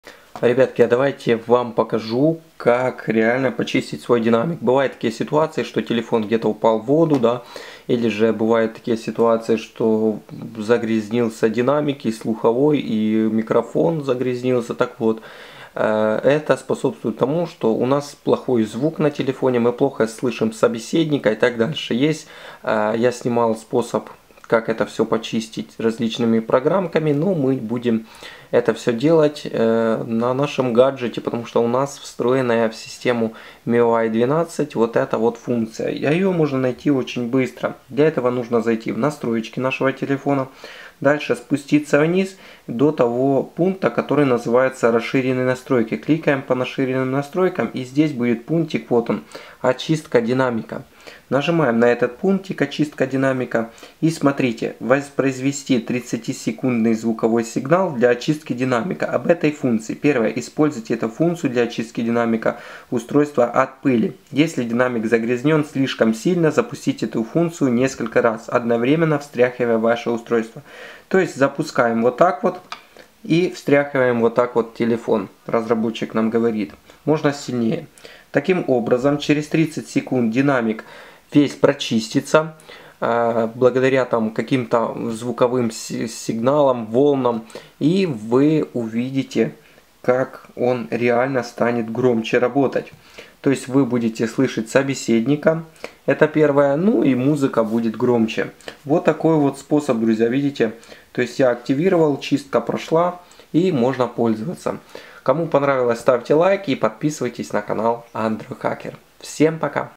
Как прочистить динамик на телефоне Xiaomi за 1 минуту!
Многие сталкиваются с такой проблемой когда на телеофне очень плохо слшно собеседника. В таком случае нужно либо менять динамик или же воспользоватся чисткой динамик, такая есть на всх телефонах xiaomi после обновления! Так вот, эта настройка действительно уникальна в своем роде, ведь динамик реально чистится. Так что кто друзья не пользовался этой настройкой, теперь у вас появился шанс опробовать ее. Надеюсь мое видео многим из вас станет полезным и интересным.